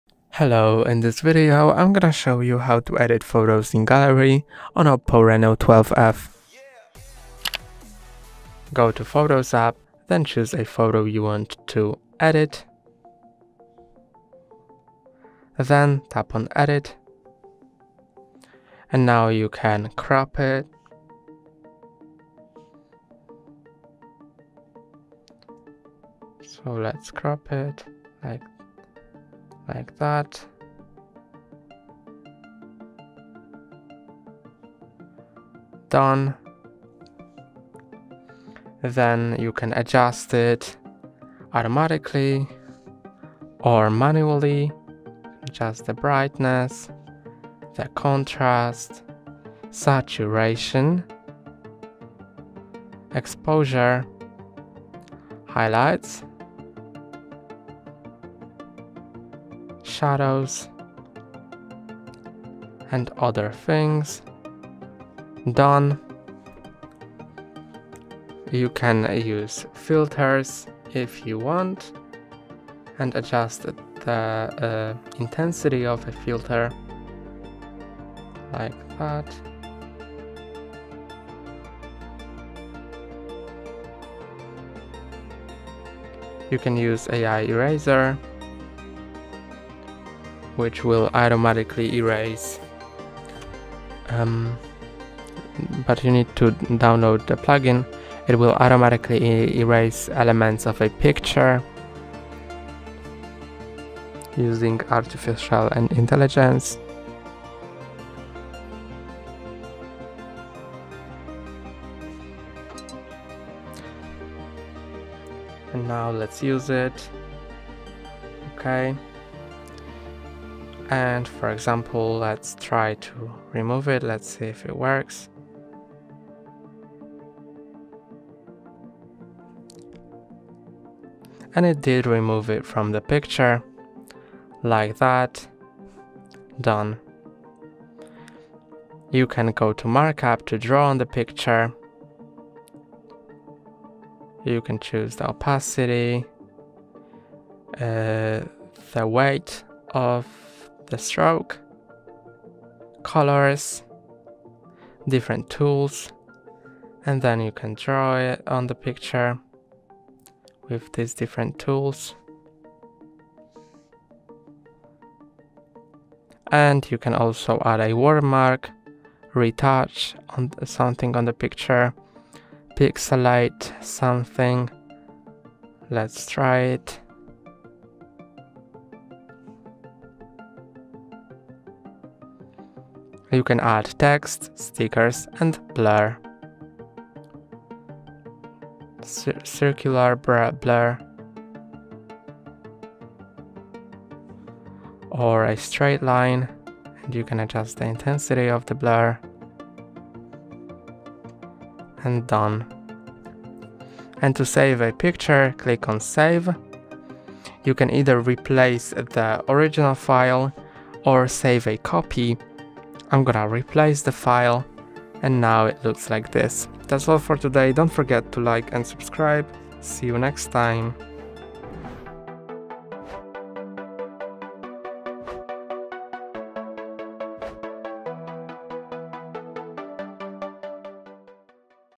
Oppo Reno 12 F - How to Edit Photos in Gallery - Step-by-Step Guide to Enhance Your Images
In this tutorial, we'll show you how to edit photos directly in the gallery app on your Oppo Reno 12 F smartphone. Editing photos allows you to enhance your images by adjusting brightness, contrast, saturation, and applying filters or effects. We’ll guide you through accessing the photo editing tools, making various adjustments, and saving your edited photos right from the gallery. Whether you want to make simple tweaks or create stunning visuals, this video will provide all the steps you need to master photo editing on your Oppo Reno 12 F. Join us as we explore how to make your pictures look their best with the built-in editing features.

Topics Covered:

How to access the gallery app and open a photo for editing
How to use editing tools like crop, rotate, and adjust brightness or contrast
How to apply filters and effects to enhance your photos
Tips for making professional-quality edits on your smartphone
How to save and share your edited photos from the Oppo Reno 12 F gallery